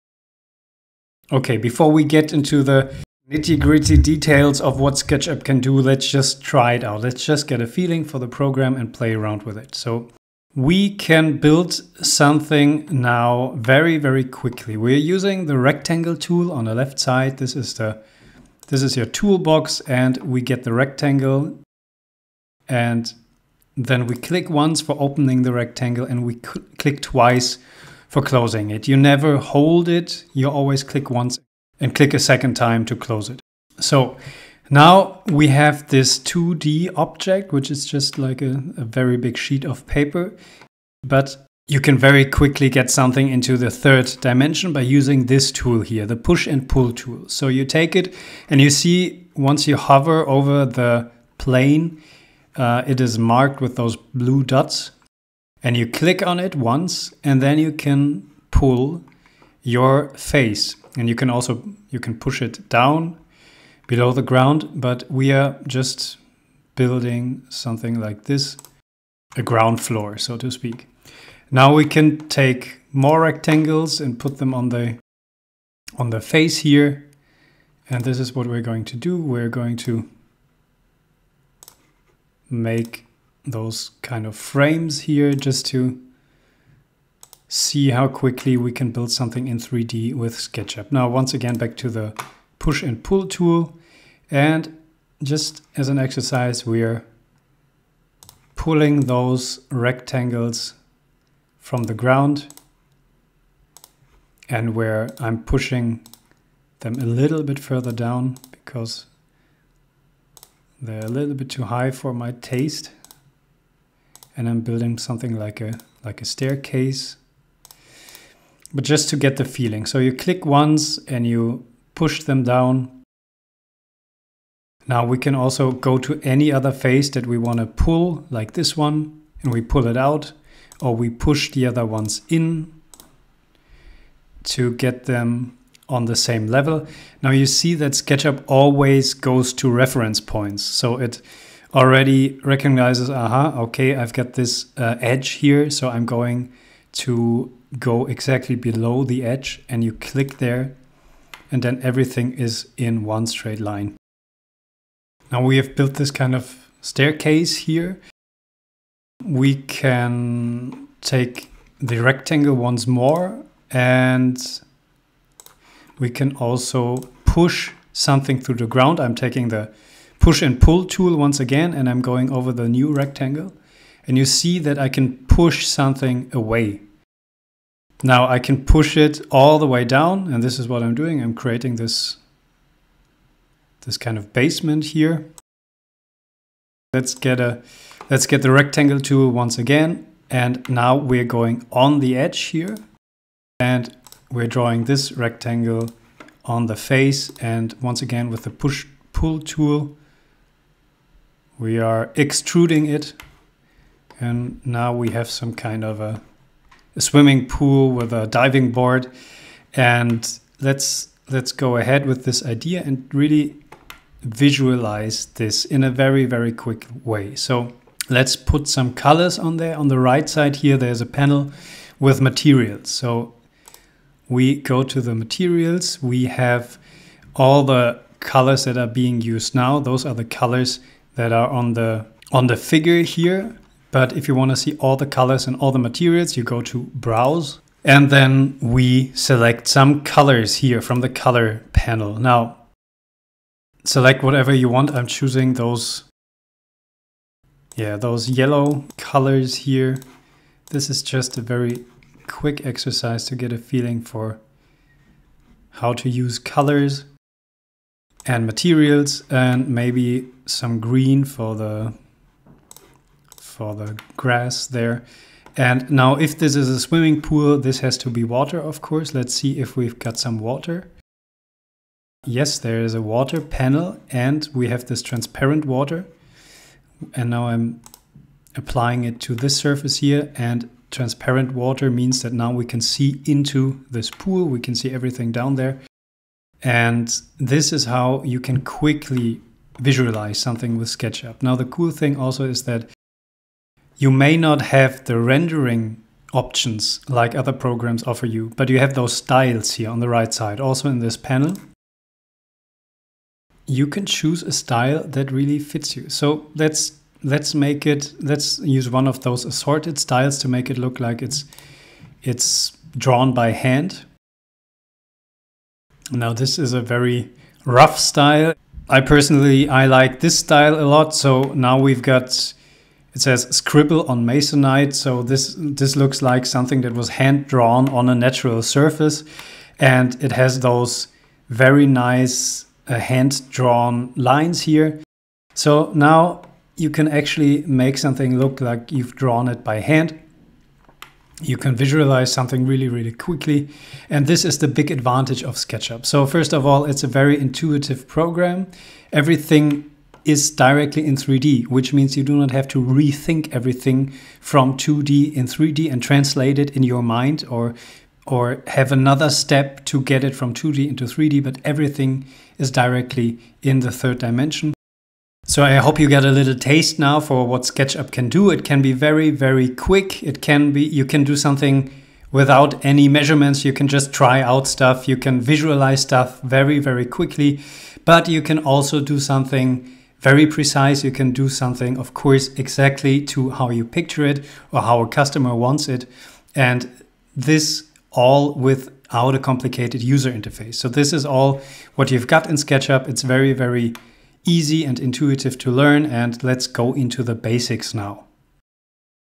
05 - Just try it: The Swimming Pool // SketchUp for Web
Before going into the specifics, let's try building something quick and easy with the rectangle and the push/pull tool.

👉 If you want to learn SketchUp, then take a look at the comprehensive SketchUp course from faber.academy on Skillshare or Udemy.